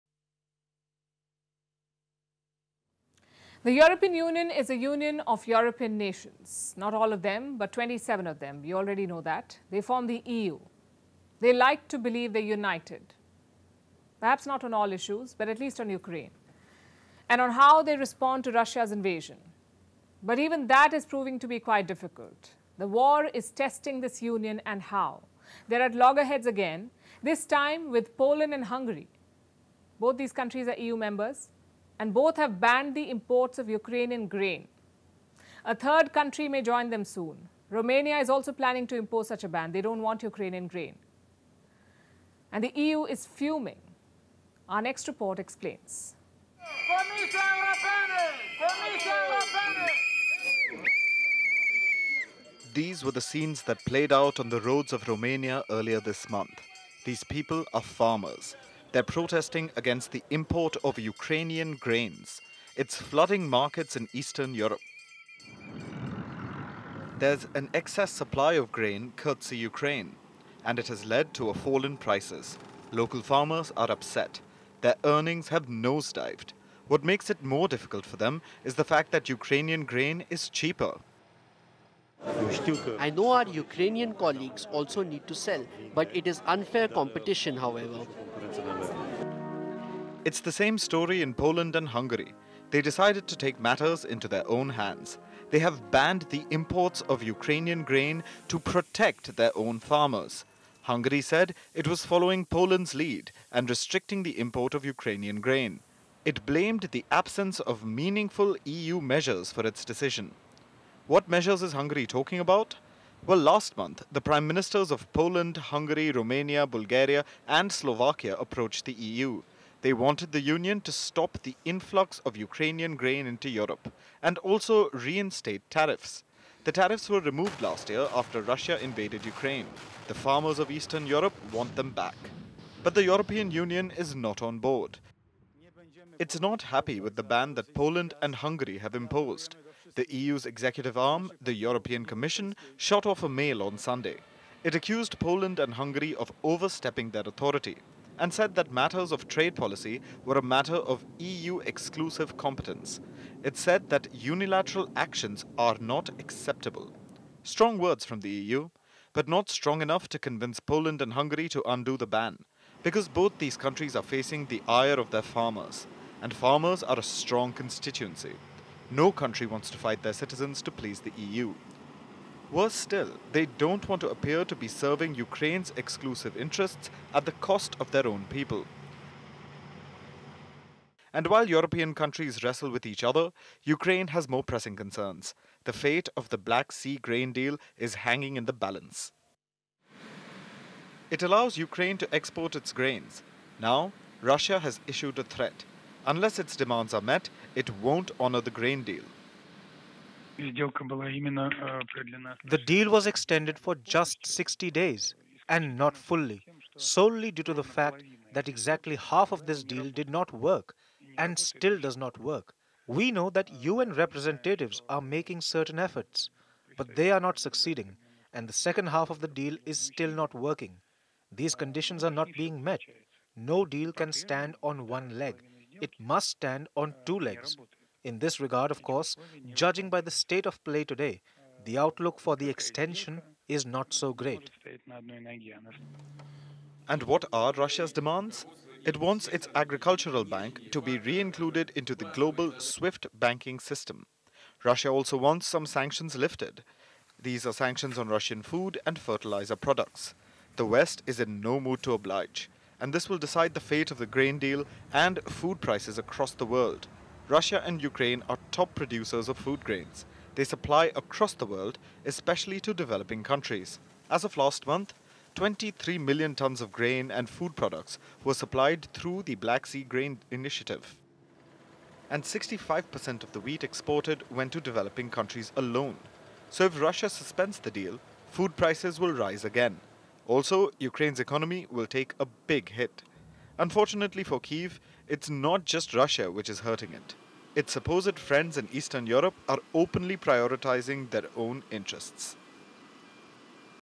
Rift in Europe: Why Poland, Hungary Banned Ukrainian Grain Imports | Vantage with Palki Sharma
The war in Ukraine is testing Europe. The EU is at loggerheads again. Poland and Hungary have banned Ukrainian grain imports. This has left Brussels fuming. Palki Sharma tells you why

Russia Ukraine War | Ukraine Grain Deal | EU | European Union | Poland | Hungary | Trending News | Firstpost